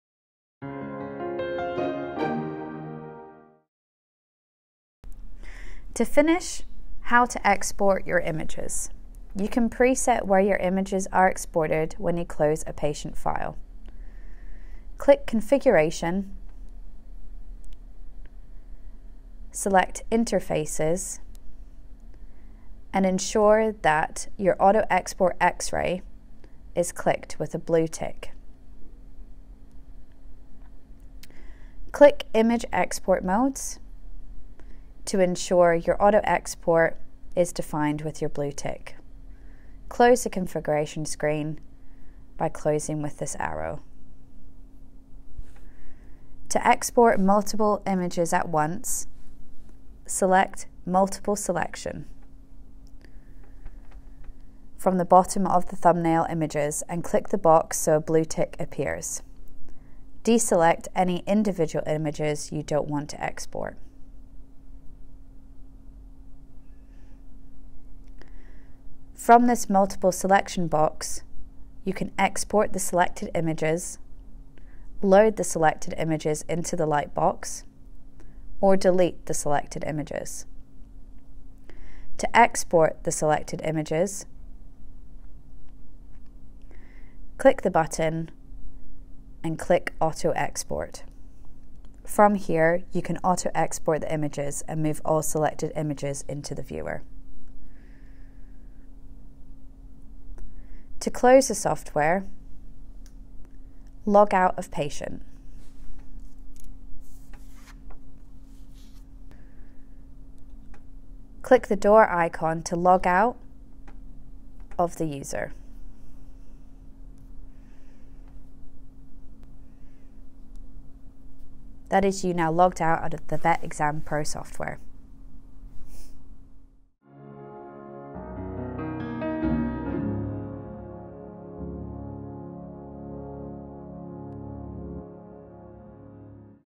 CR 7 Vet - Dental X-ray Scanner and Plate: Export and Close Software - video 7
Claire-Louise, account manager from IMV imaging takes you through the details of how to export images and examinations before showing you how to exit down the software.

This equipment is provided by IMV imaging, specialists in veterinary imaging equipment, learning, service and support.

iM3 Vet Dental X-Ray
CR 7 Vet Image plate, x-ray scanner and Vet-Exam software for Veterinary Medicine.